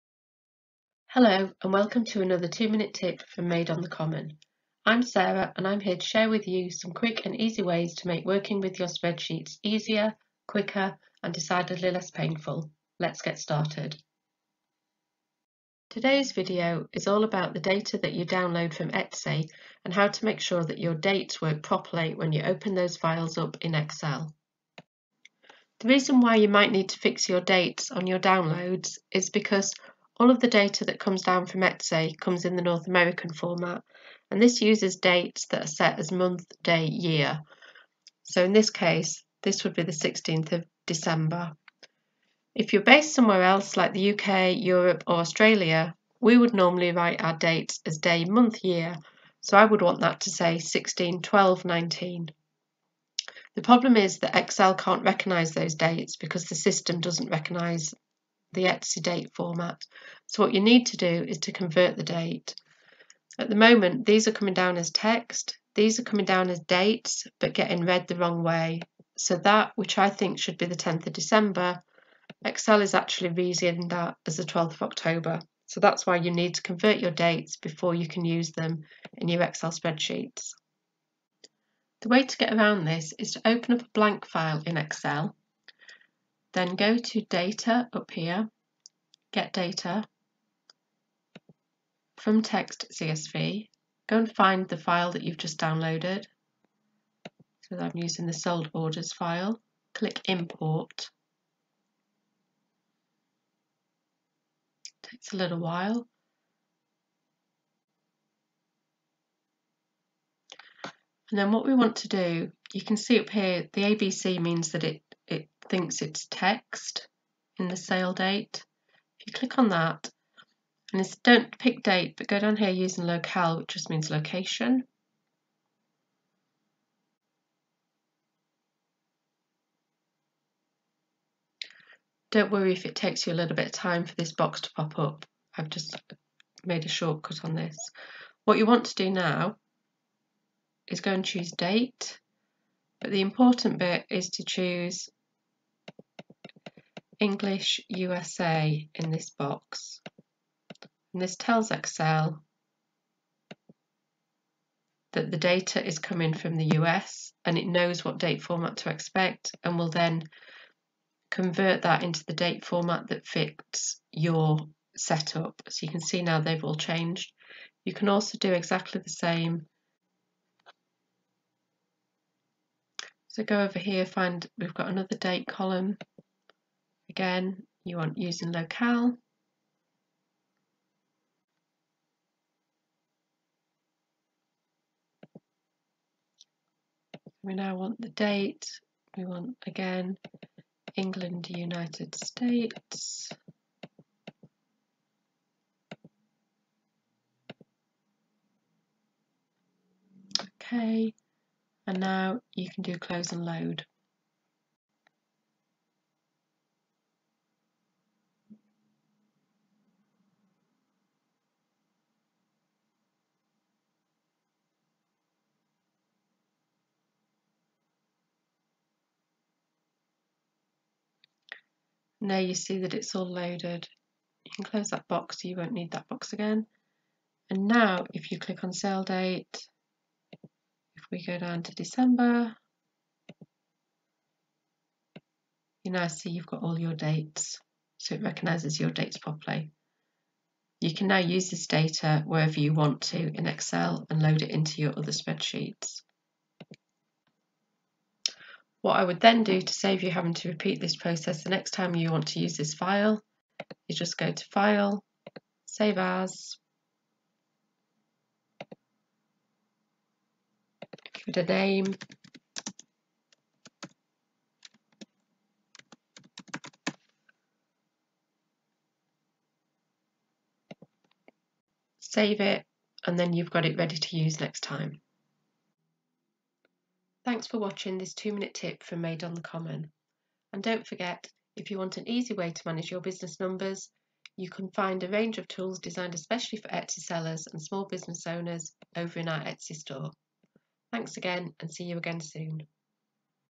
2 Minute Tips: How to Fix the Dates in your Etsy Downloads
2 Minute Tips to help you get the most of your spreadsheets and Etsy data.

How to Fix Your Dates
If you're based outside North America then you may find that your dates don't work properly in your Etsy data downloads.
Find out how to fix them with this easy hack.

For an easy way to organise your business numbers check out our automated bookkeeping templates for Etsy Sellers and small business owners.